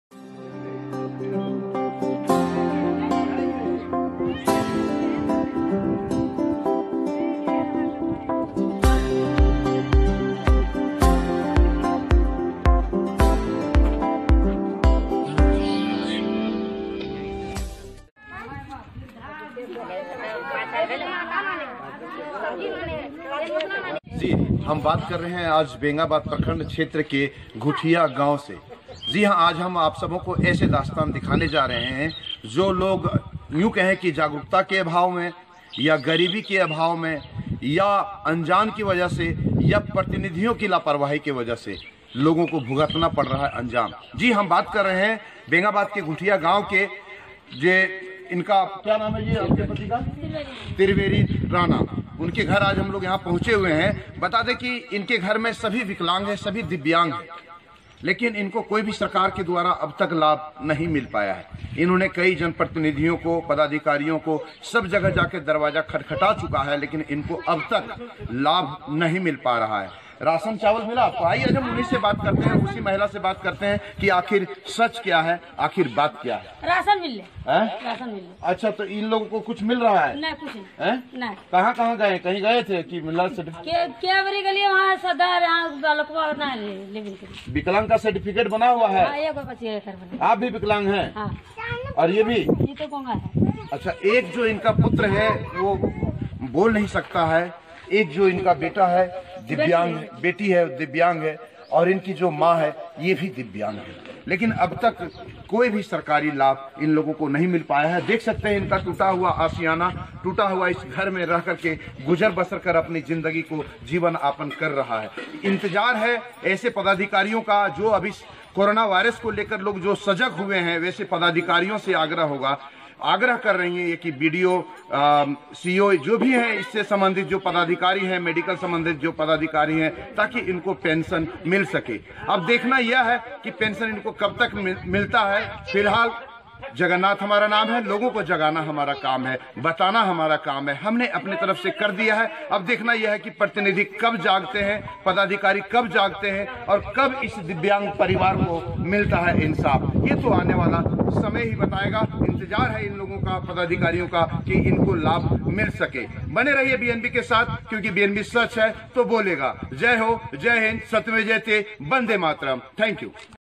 दिव्यांग है पूरा परिवार मदद की लगाई है गुहार किसी ने नही सुनी अब तक पुकार देखें BNB में आख़िर क्या है
दिव्यांग है पूरा परिवार मदद की लगाई है गुहार किसी ने नही सुनी अब तक पुकार देखें BNB में आख़िर क्या है। माजरा।